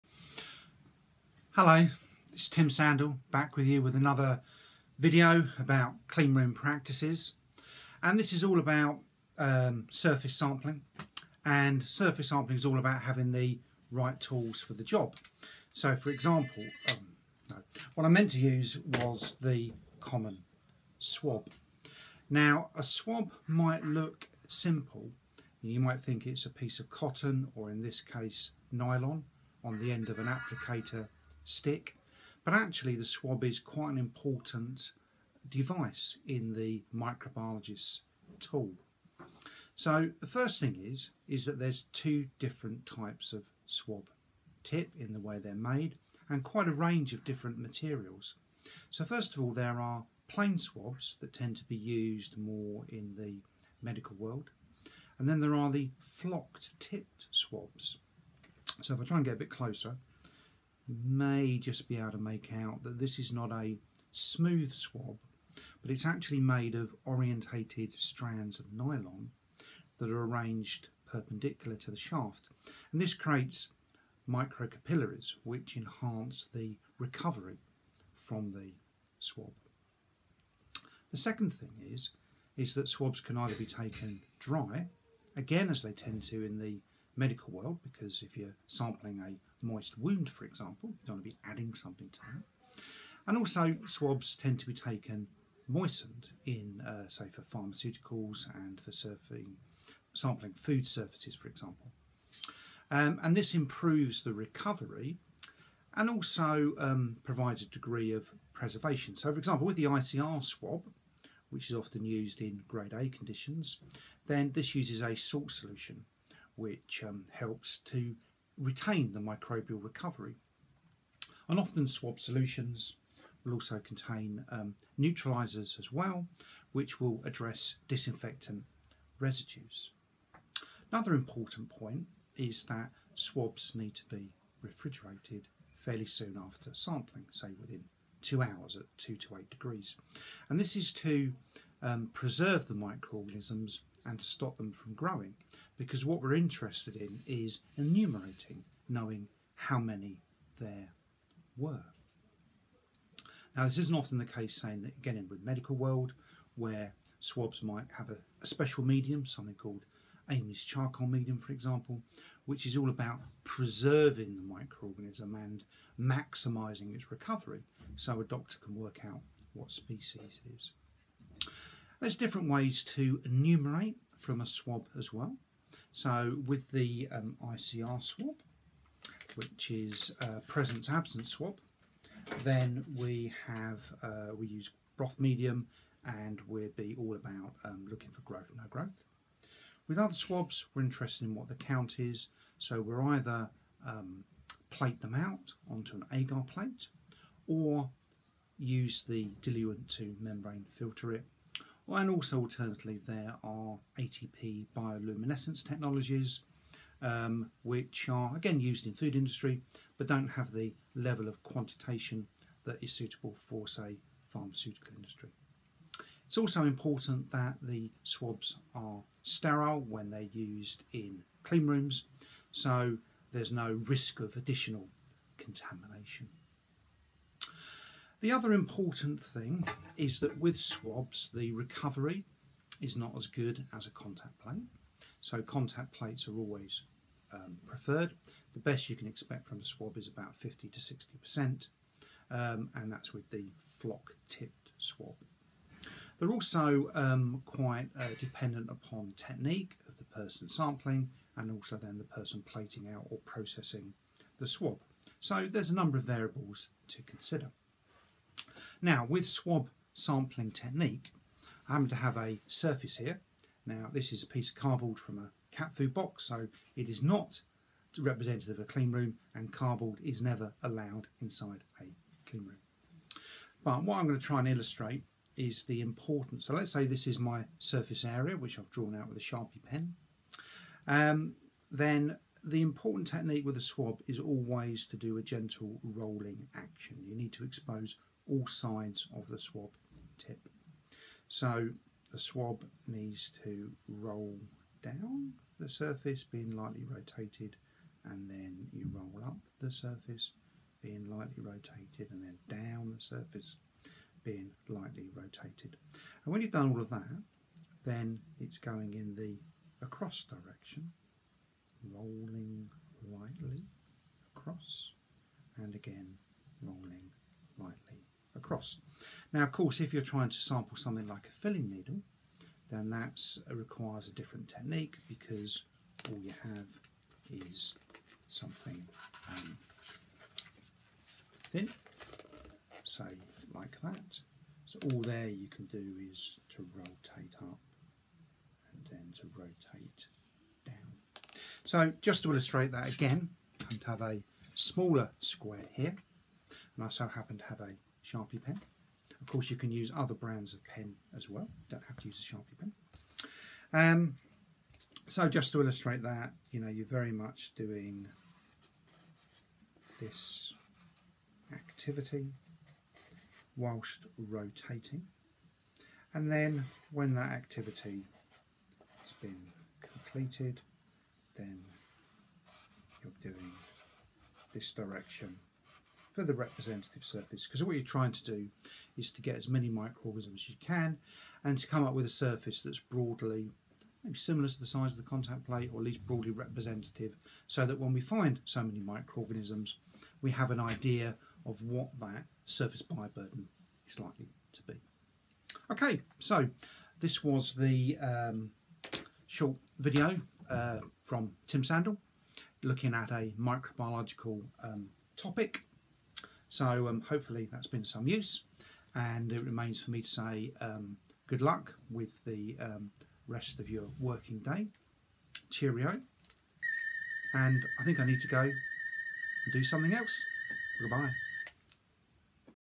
How to take microbiological swabs from surfaces
Tim Sandle looks at surface sampling using swabs from cleanroom surfaces